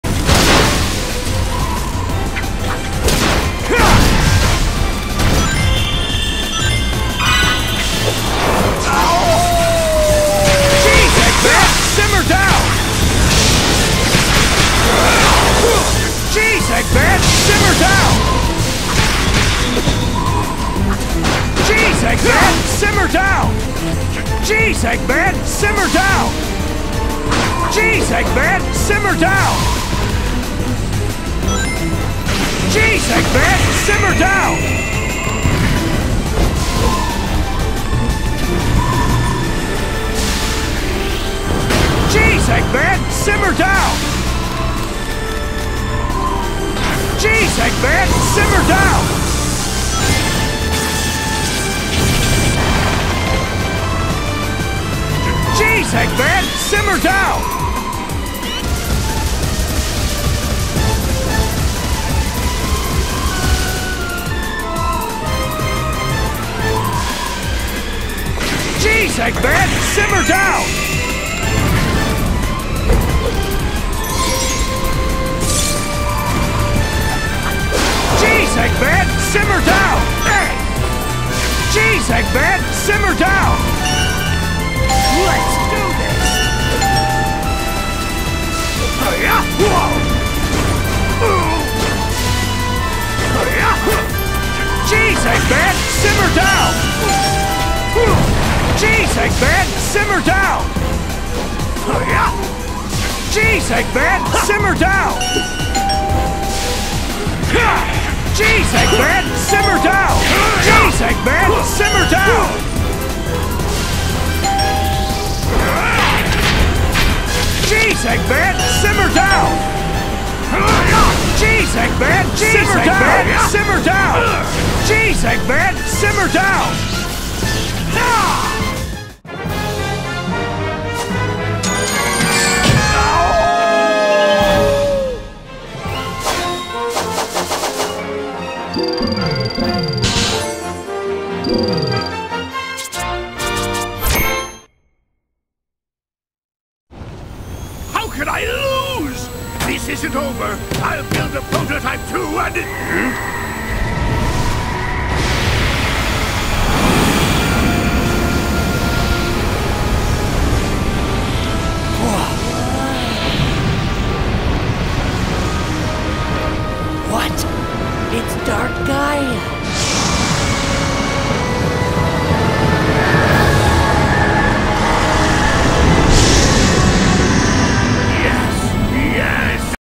Geez Eggman, Simmer down!
Was playing the game till the audio for Eggman glitched up